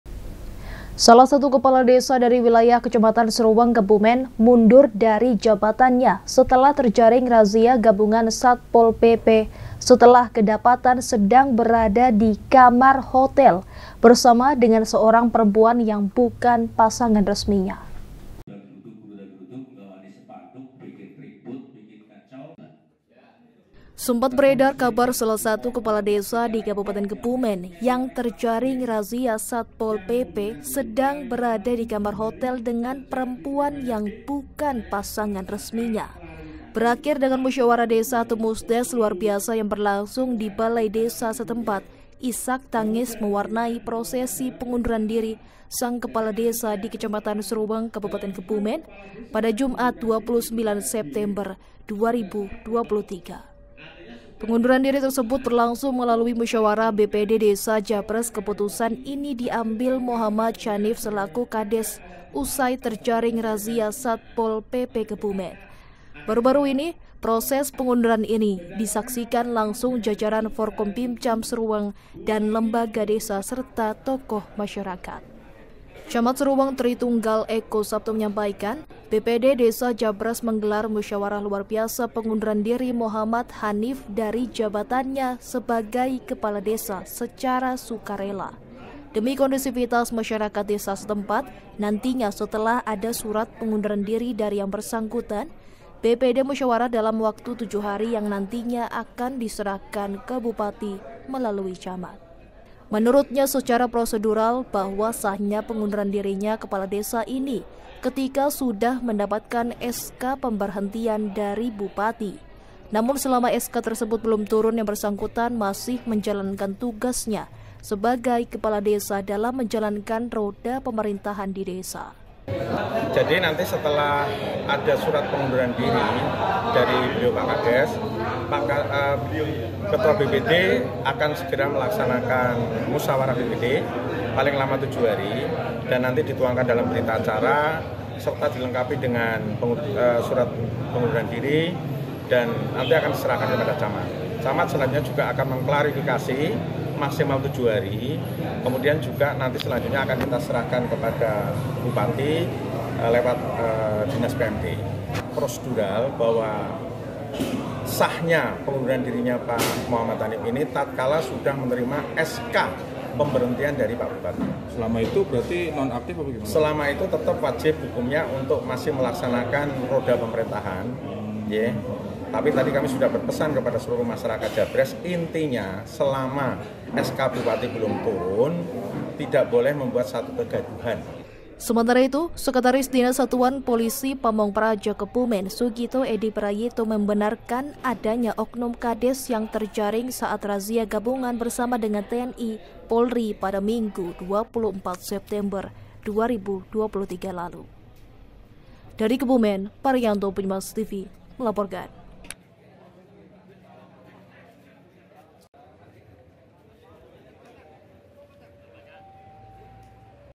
KADES JABRES MUNDUR DARI JABATANNYA SETELAH TERJARING RAZIA SATPOL PP
SALAH SATU KEPALA DESA DARI WILAYAH KECAMATAN SRUWENG, KEBUMEN, MUNDUR DARI JABATANNYA SETELAH TERJARING RAZIA GABUNGAN SATPOL PP, SETELAH KEDAPATAN SEDANG BERADA DI KAMAR HOTEL BERSAMA DENGAN SEORANG PEREMPUAN YANG BUKAN PASANGAN RESMINYA.